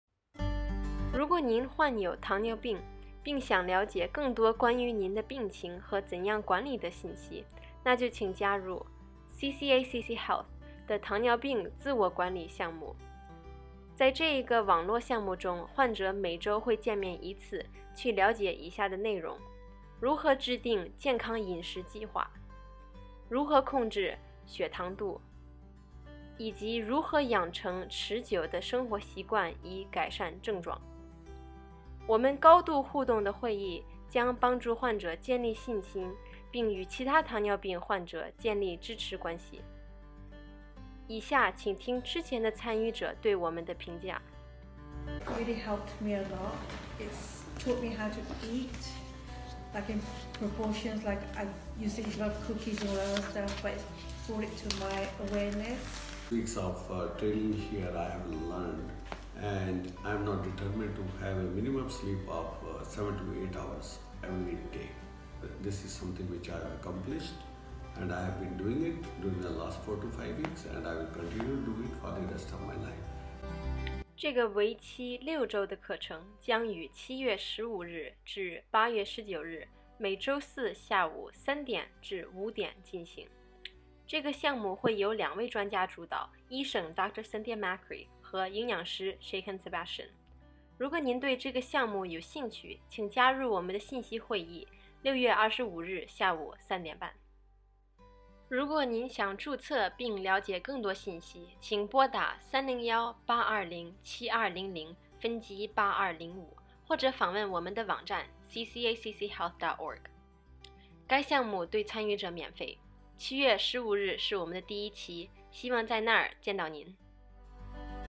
糖尿病自我管理系列课程介绍
如果您患有糖尿病并想了解更多有关您的病情和怎样管理的信息，那就请加入 CCACC Health 的“糖尿病自我管理计划！”

在这个网络项目中，患者每周会见面一次，去了解以下的内容：如何制定健康饮食计划、如何控制血糖度，以及如何养成持久的生活习惯以改善症状。

我们高度互动的会议将帮助患者建立信心并与其他糖尿病患者建立支持关系。

以下请听之前的参与者的评论
这个为期 6 周的课程将于 7 月 15 日至 8 月 19 日，每周四下午 3 点至 5 点进行。

这个项目会由两位专家主导，医生Dr. Cynthia Macri和营养师Shahin Sebastien

如果您对这个项目有兴趣，请参加我们的信息会议，六月25日下午3:30.

7月15日是我们的第一期，希望在那里见到您！